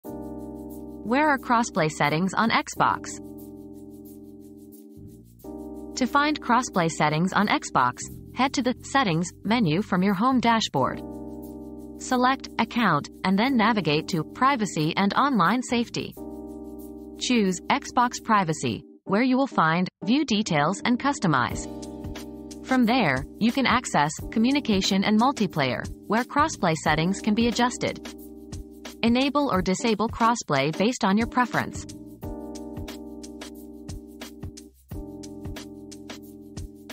Where are crossplay settings on Xbox?
Xbox Crossplay Settings Tutorial • Xbox Crossplay Guide • Learn how to adjust crossplay settings on Xbox in just a few simple steps. Customize your multiplayer experience by enabling or disabling crossplay with this easy tutorial.